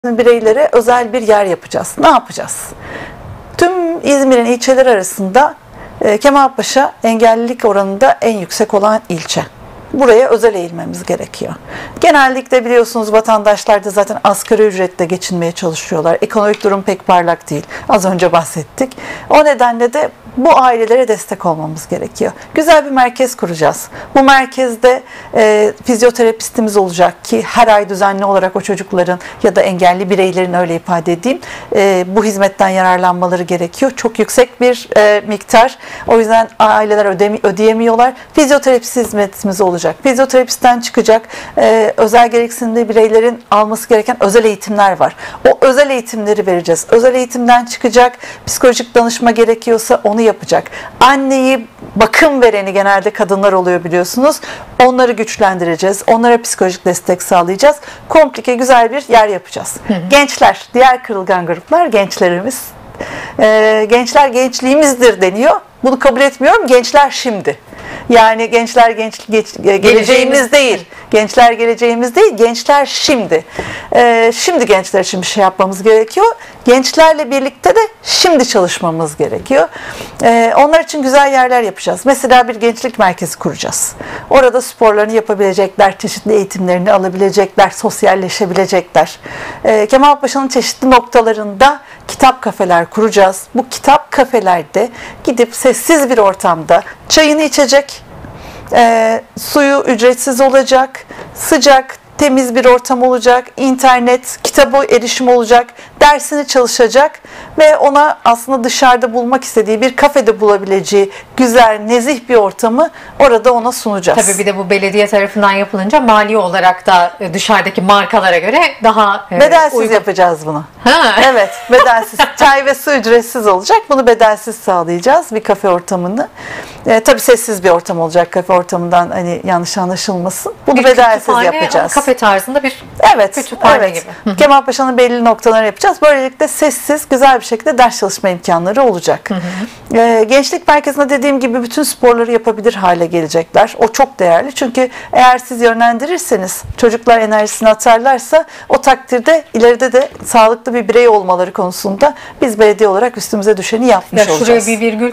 "Gençler geleceğimiz değil. Gençler şimdi!" / Arzu Külahçıoğlu Altıntoz
GÜNÜN KONUĞU

AV. ARZU KÜLAHÇIOĞLU ALTINTOZ - CHP KEMALPAŞA BELEDİYE BAŞKAN ADAY ADAYI